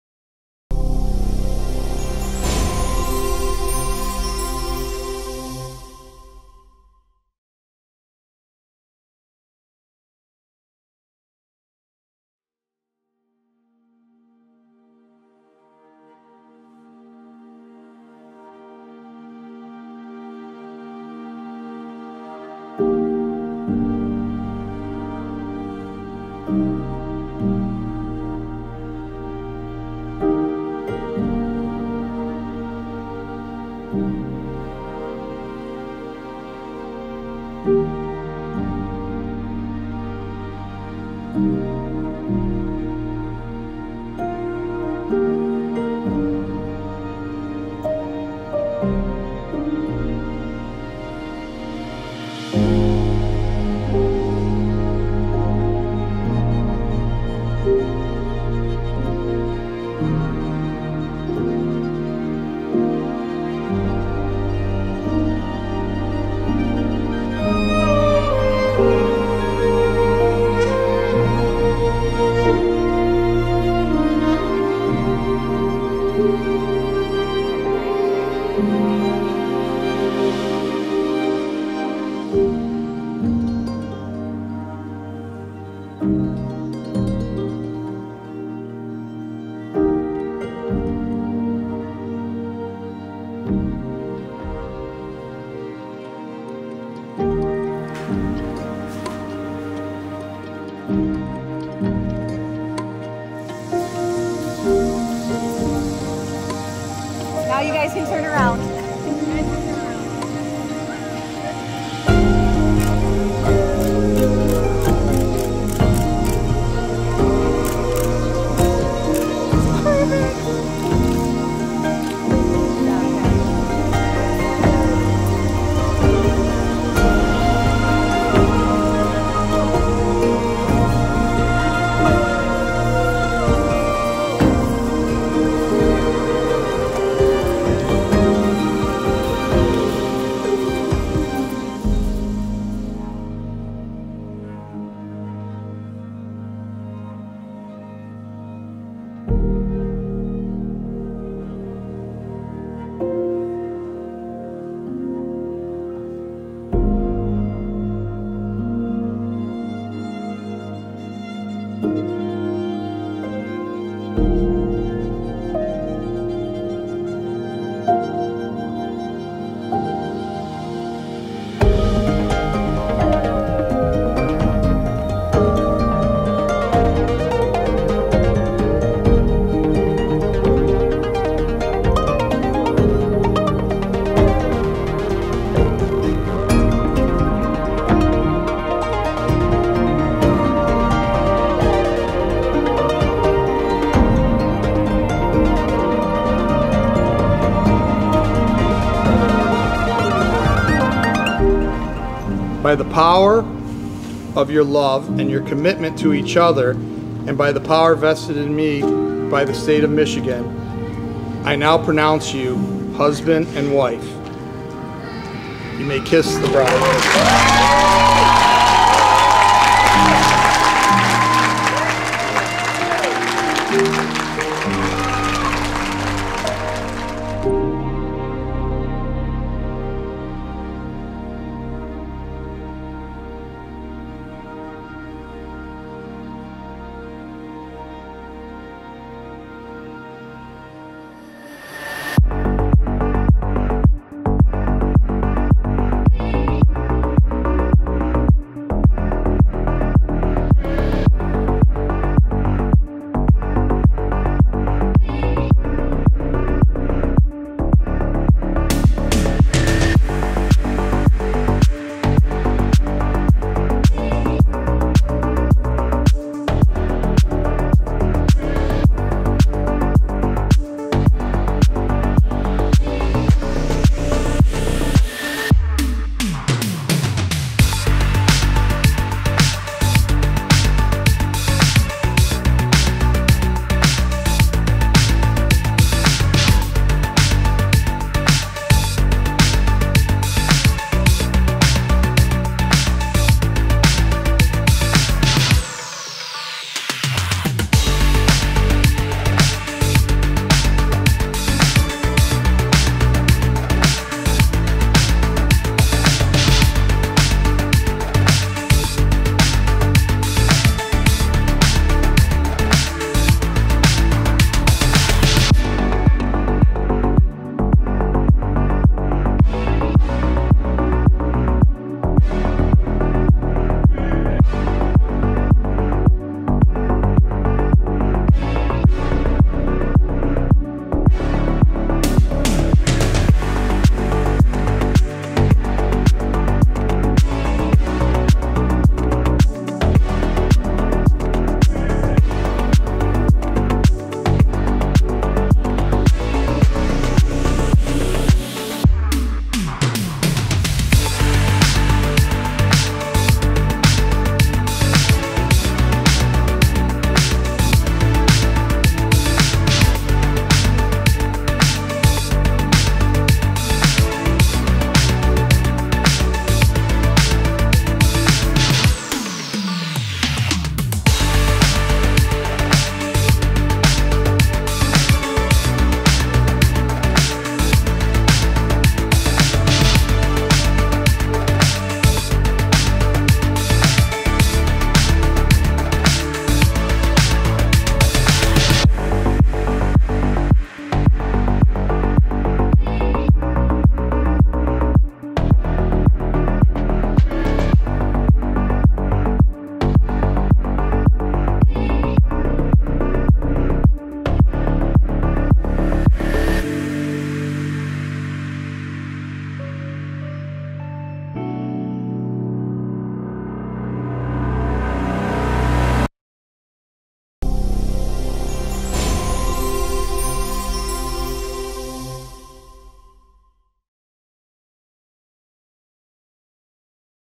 09 16 2022 Lisa & Chris Highlight
Lisa & Chris had their beautiful wedding ceremony and reception at Andiamo Italia in Warren Michigan!  JD Entertainment was there to provide DJ & Video services.  Make sure you check out their 1st look and their 1st dance in one of the best weddings videos near Detroit.